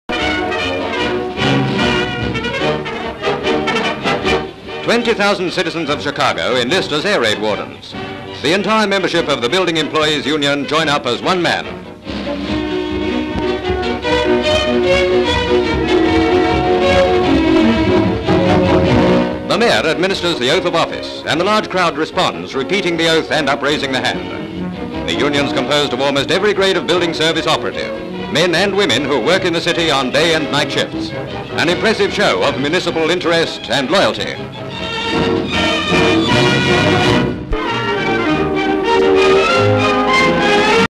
Wardens' Mass Enlistment Dublin Issue (1942)
Titles read: "WARDENS' MASS ENLISTMENT" (Dublin issue).

Chicago, Illinois, United States of America.

In a large hall we see 20,000 members of the Building Employers Union enlisting as Air Raid Wardens at the same time.  The Mayor of Chicago administers the Oath of Office and the large crowd responds, holding up their hands (oath is not heard).
 FILM ID:1314.2